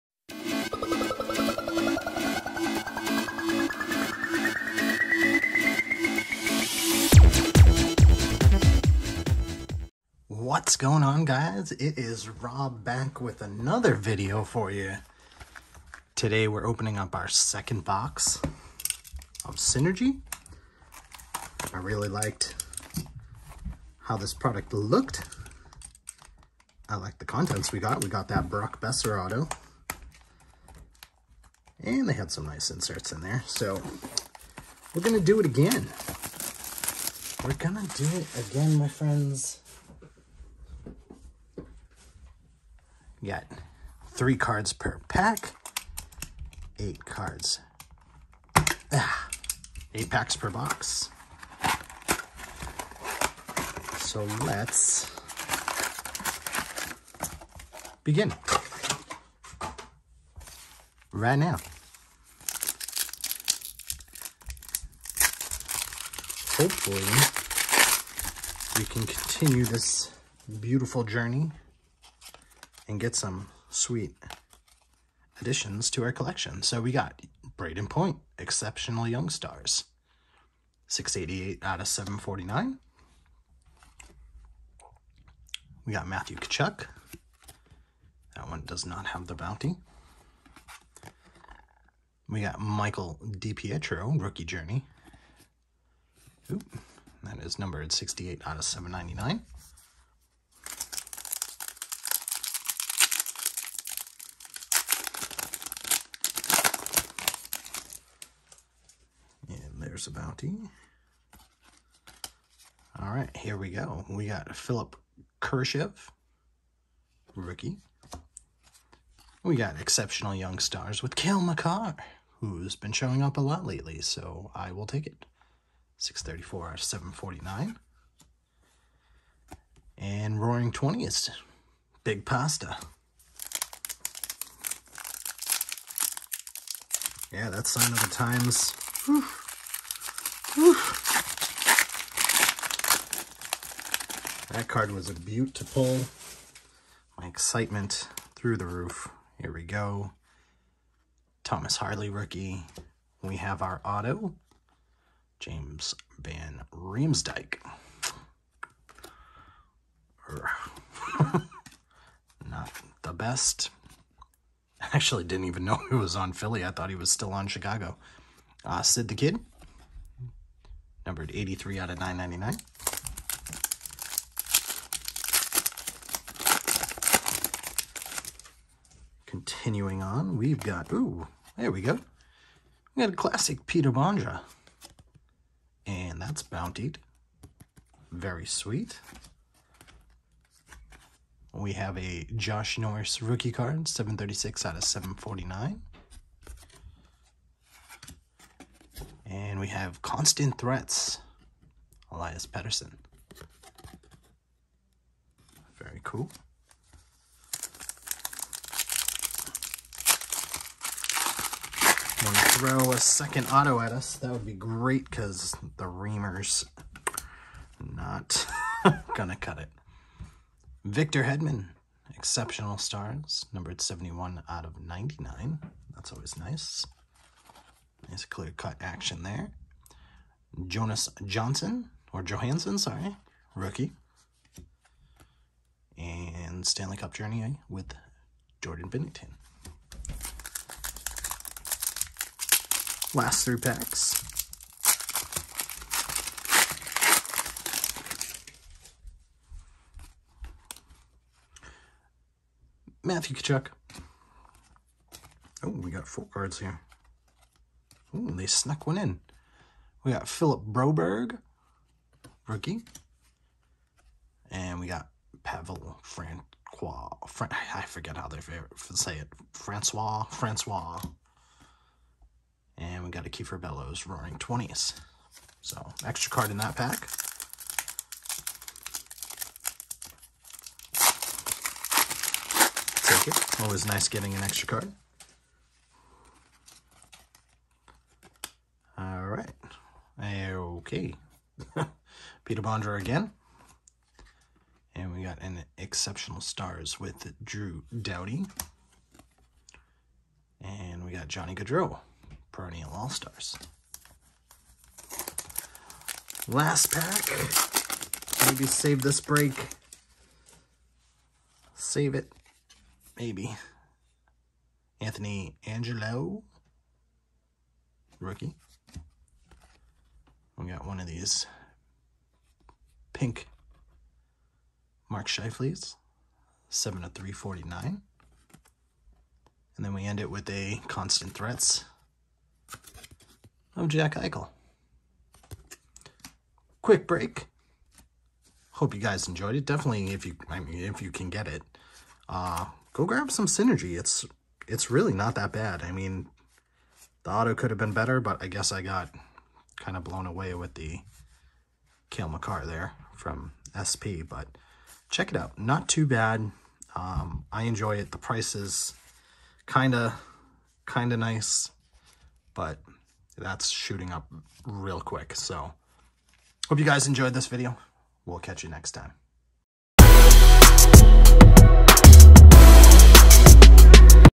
Synergy Box 2
We open up box number 2 of 20/21 Synergy!

Let's keep the conversation going!